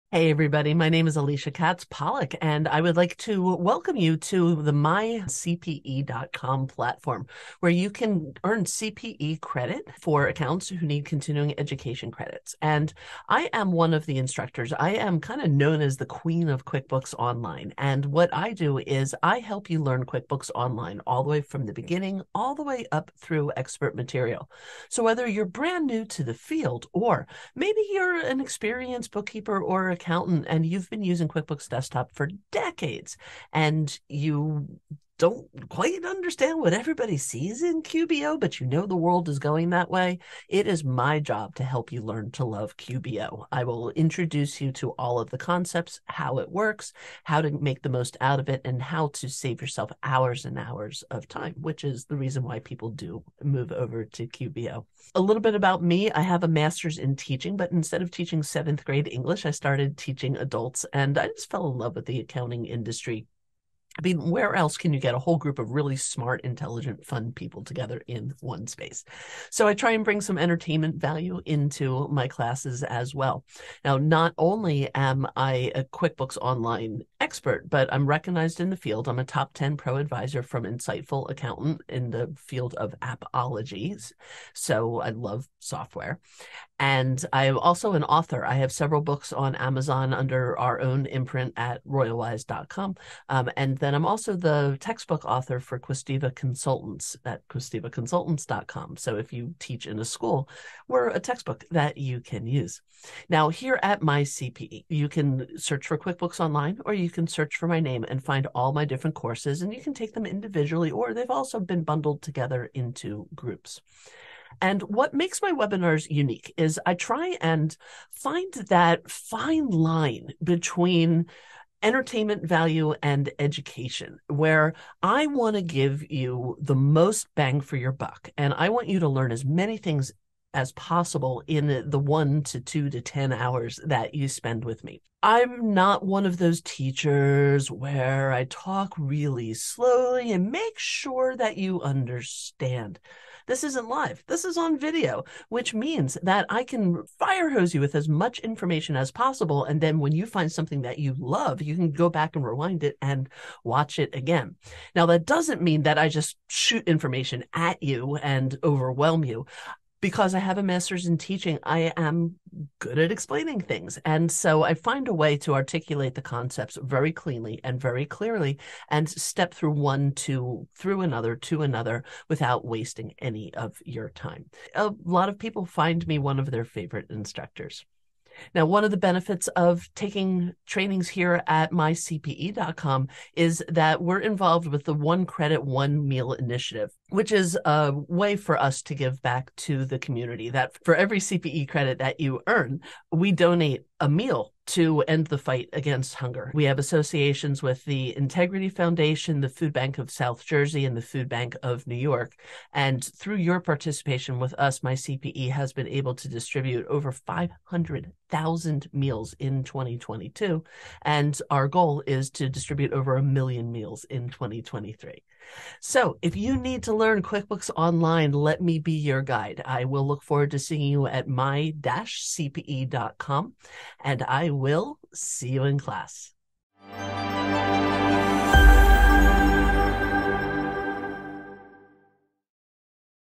Meet the Educators | Alicia Pollock | myCPE
Meet our Educator Alicia Pollock #1 QB Expert. Alicia a member of the Intuit Trainer/Writer Network, and named Top 10 ProAdvisor by Insightful Accountant. With a Masters in Teaching, a collection of certifications, several published books, & decades of business consulting experience, Alicia is a skilled & patient QuickBooks coach, having trained 60,000+ professionals with 2,000+ reviews.

All of the Latest courses on QuickBooks are available under the myCPE Unlimited Access Plan at $199.